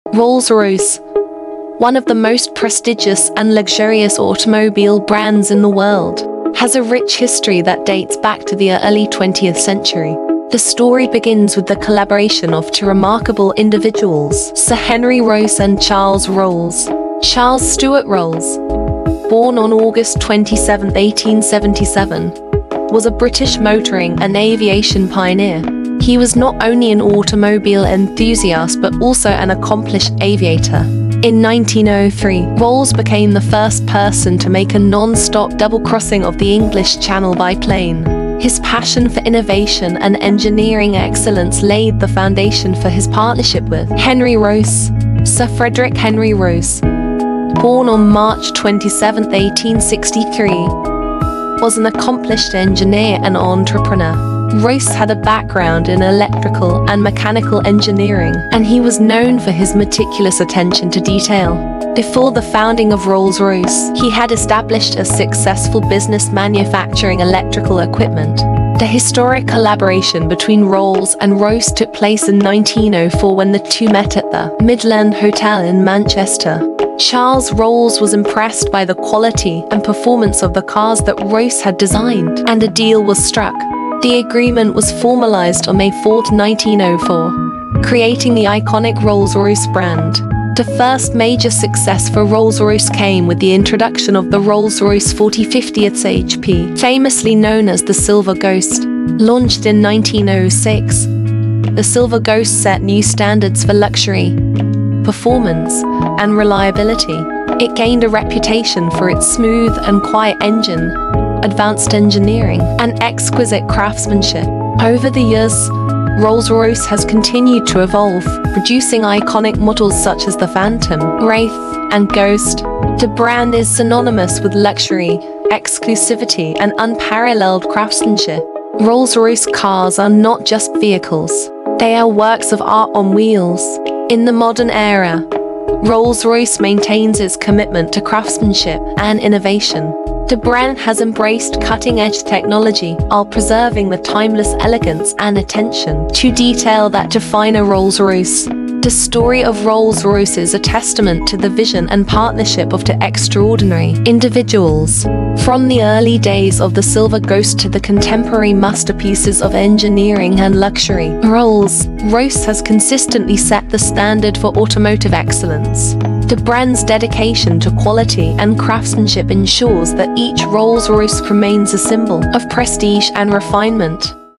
Did You Know The History Of Rolls Royce?
Did you know that Rolls-Royce, one of the most prestigious and luxurious automobile brands in the world, has a fascinating history dating back to the early 20th century? Join us as we uncover the story of two remarkable individuals, Sir Henry Rolls and Charles Rolls, and how their collaboration gave birth to an iconic brand.